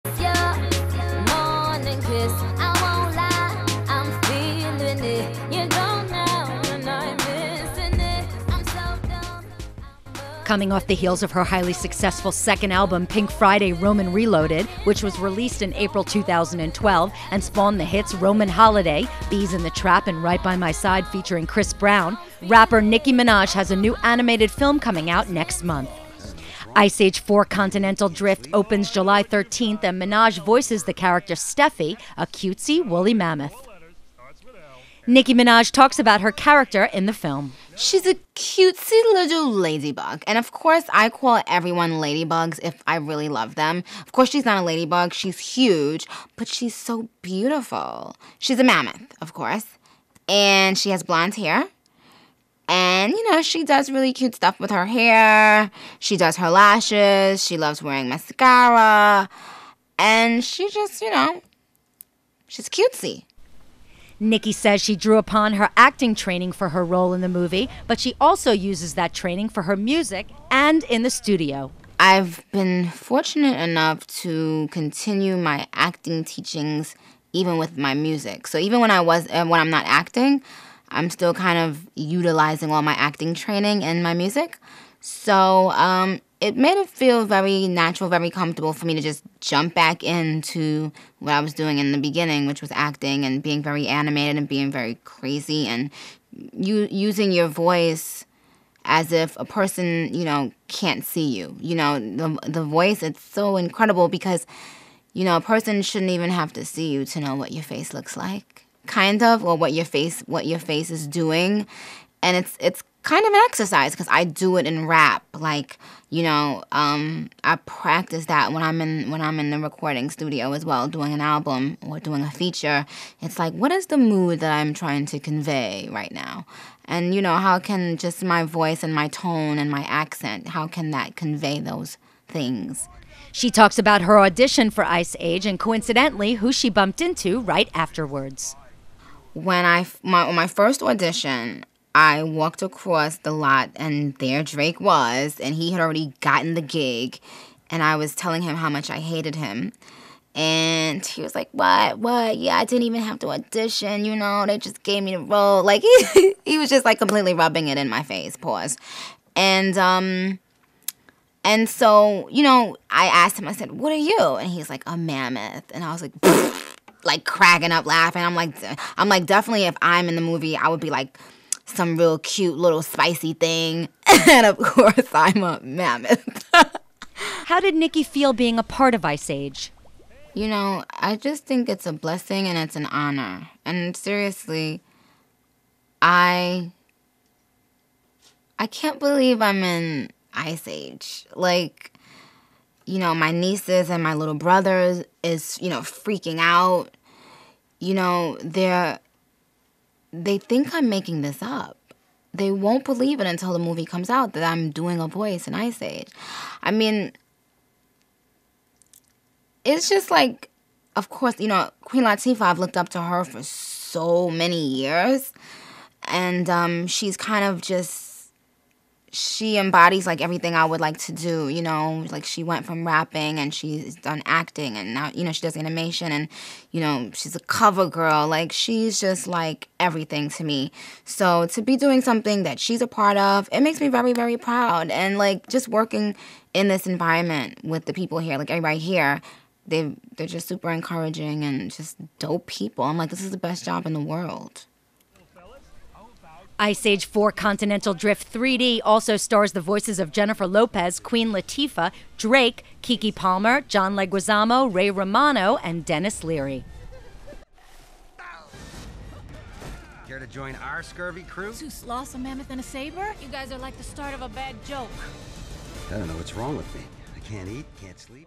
NICKI MINAJ A MAMMOTH, FOLLOWS DRAKE LEAD, SAYS LATIFAH ROLE MODEL
Nicki Minaj is part of an all star cast in the new film Ice Age: Continental Drift, which hits theaters next month. Nicki talks about the movie, her acting training, Drake, and how she looks up to Queen Latifah.